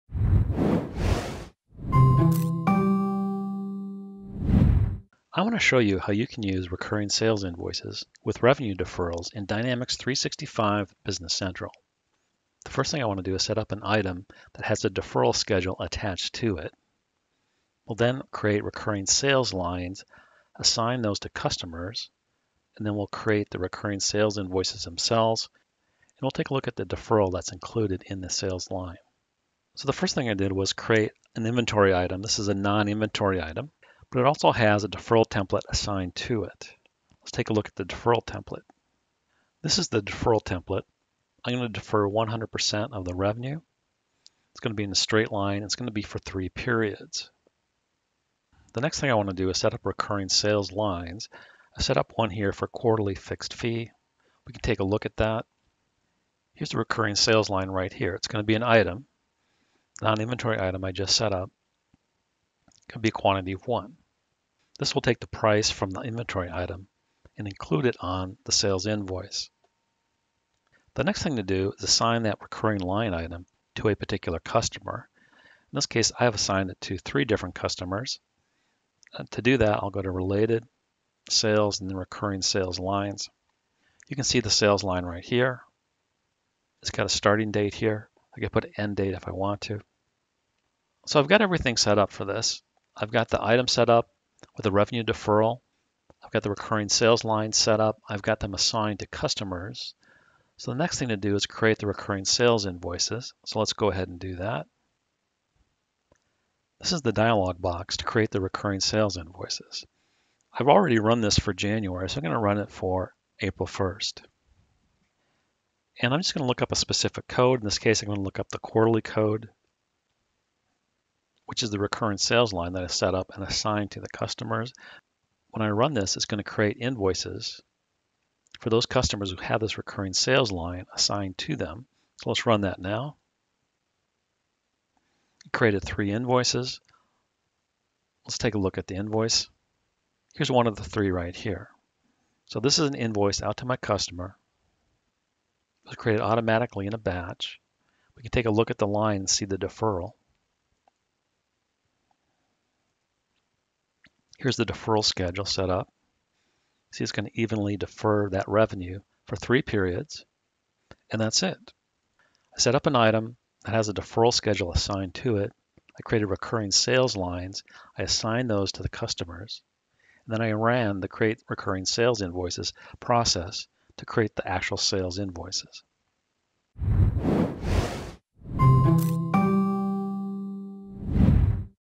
Recurring Sales Invoices with Revenue Deferrals in Dynamics 365 Business Central
Shows how to easily set up recurring sales invoices in D365 Business Central. This example shows quarterly invoices that have a 3 month revenue deferral schedule.

Covers Item Setup with Deferral, Recurring Sales Lines, Creating Recurring Sales Invoices.